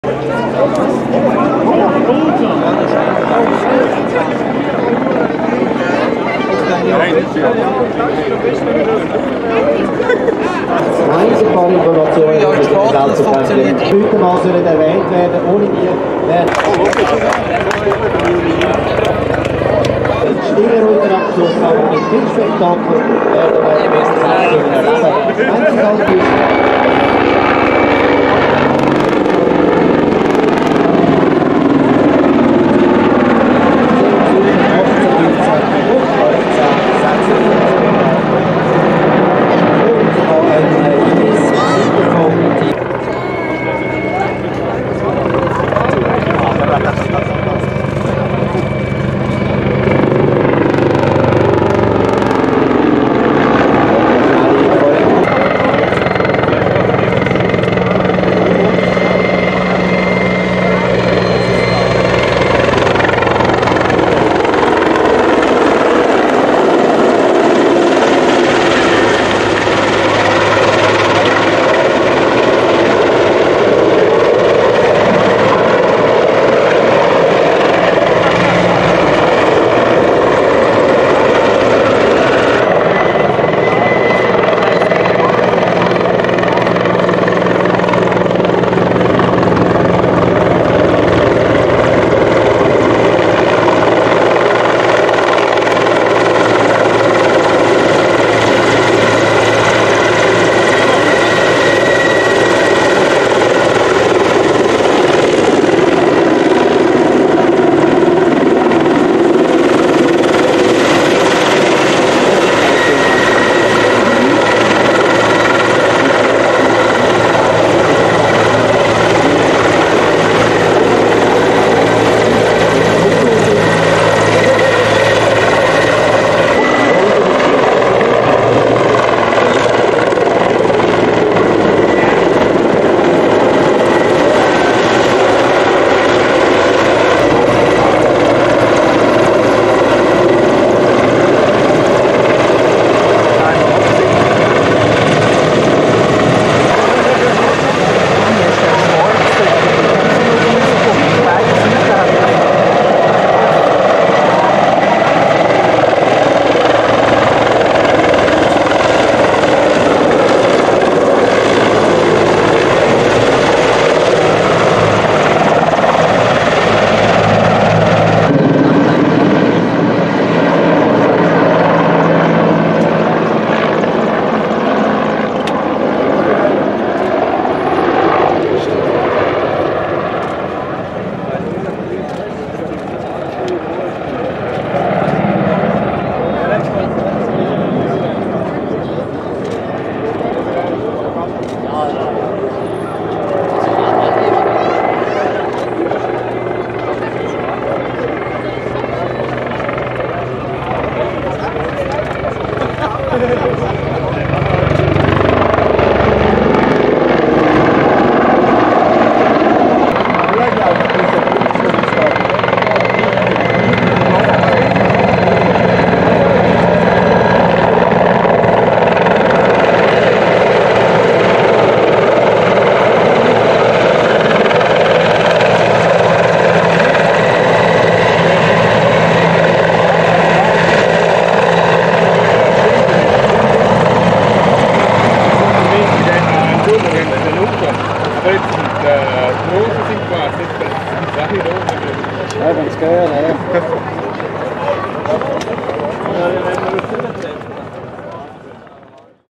Mi-35 (Mi-24V) at Zigermeet 2019
Mi-35 (Mi-24V) from Czech Airforce at Zigermeet, Mollis Switzerland, August 17 2019.